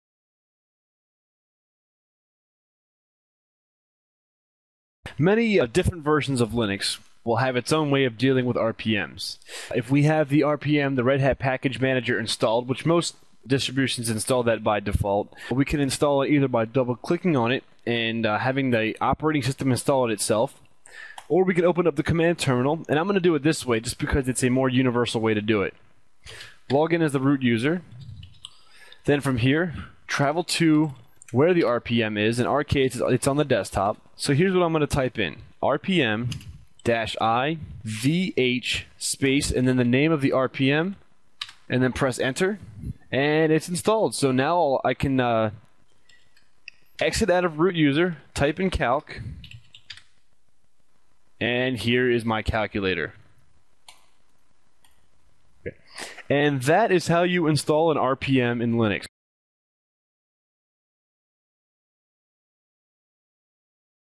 Linux GUI Calculator-19- How to Install RPM
/* WiBit.Net Video Tutorial */
Title          : Linux GUI C++ Project
Num Videos     : 19
Lesson Length  : 2 hours 10 minutes 52 seconds
Resolution     : 800 x 600
Date Recorded  : 5/21/2006
Narrator(s)    : Kevin Willett
OS:            : SuSE Linux 10.0
Software       : QT Designer, KDevelop
Description    : A project was assigned for an Operating Systems class in
                 which we had to create some application that handled
                 operating system duties. My group chose to create a Linux
                 task management application, but none of us had any GUI
                 experience in Linux. Over the course of the next few days I
                 created these videos to show everyone a way to do it so that
                 we could work independently. The test project is a simple GUI
                 calculator that performs basic arithmetic. These videos start
                 with nothing but a blank text document and ends with a fully
                 functional calculator. Package and installation videos are
                 also included. If you're not a hard core coder these videos
                 will bore you to tears! But there is some great information